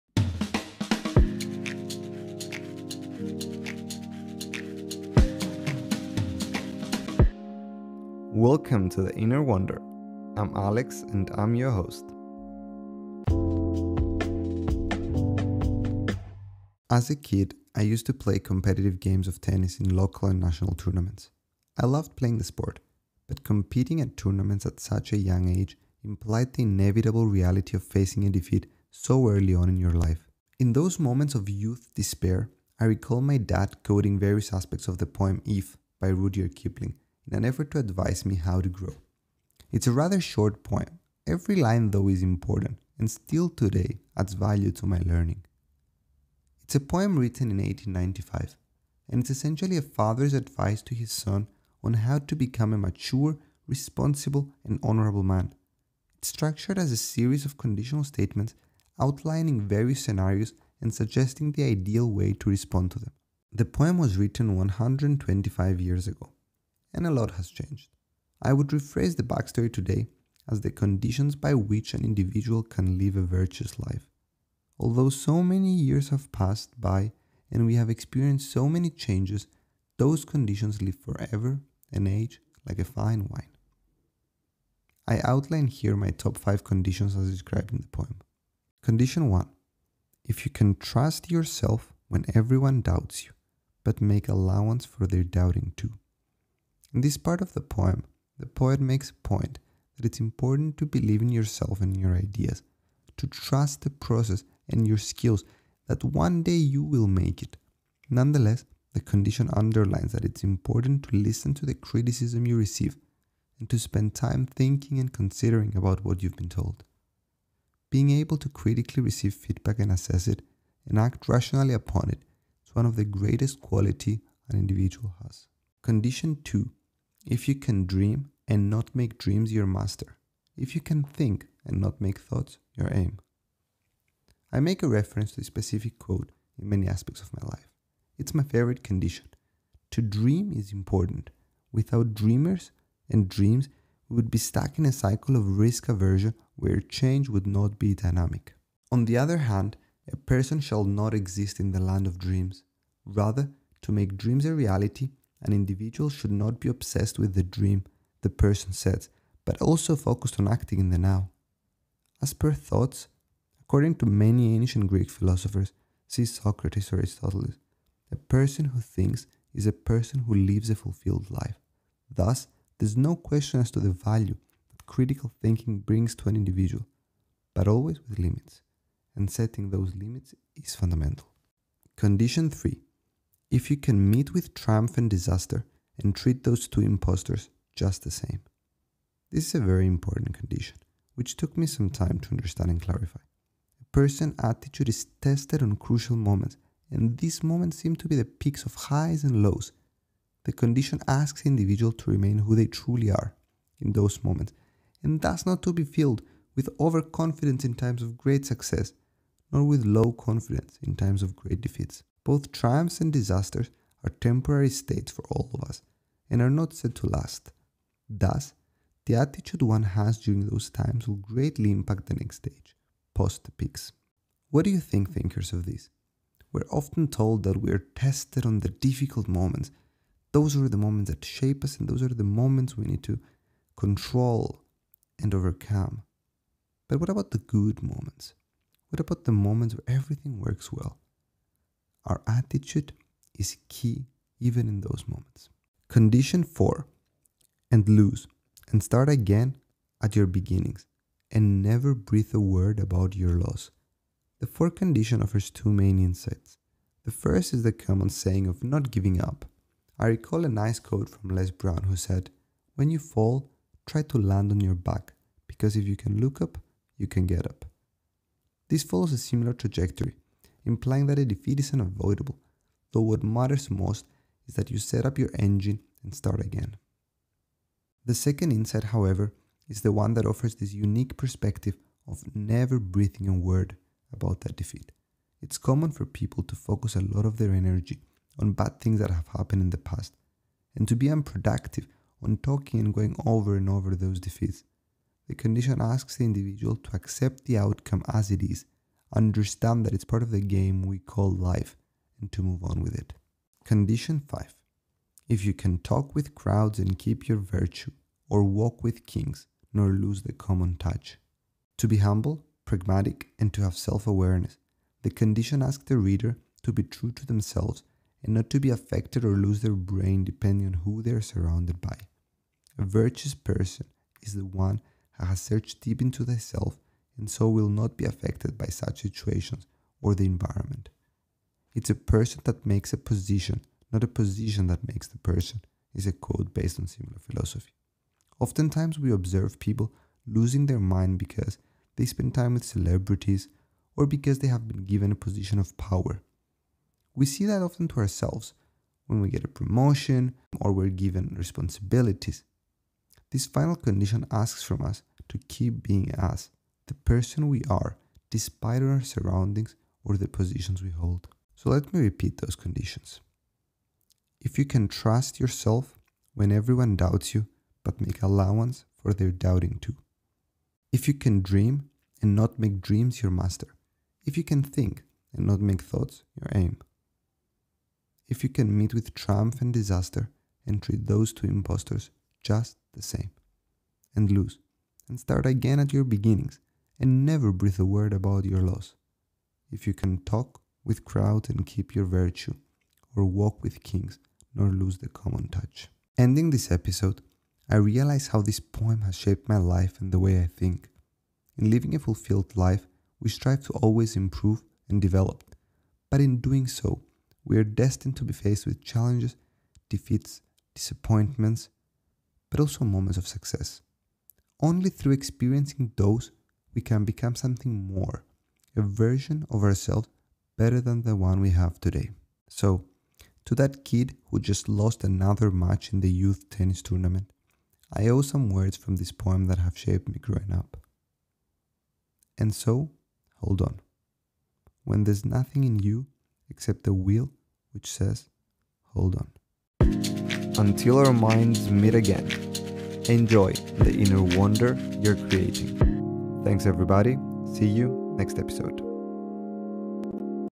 Ep5: "IF" you can trust yourself when everyone doubts you + 4 more conditions for a fulfilling life!
(Follow Inner Wonder on Instagram!)
🌏 Join me in this episode of The Inner Wonder, as we uncover 5 conditions to live a fulfilled life, given by Rudyard Kipling in his poem If!
It is a poem that lives through generations and offers advice to all of us on how we can all live a more fulfilled life. Conditions are hard to face and even harder to apply - but a challenge, that if accepted can all make us reach new highs!

💭 Additional resources that can be used as your Muses for further exploration!
1️⃣ "The Road Not Taken" by Robert Frost (p.s. The first ever article I published was on this poem! Check it out on Medium!
2️⃣ "Good Will Hunting" An old-time favourite movie, which if you haven't done so - stop what you are doing and watch! Offers perspectives on life decisions!
3️⃣ "The Bucket List" - I know, when he died, his eyes were closed but his heart was open!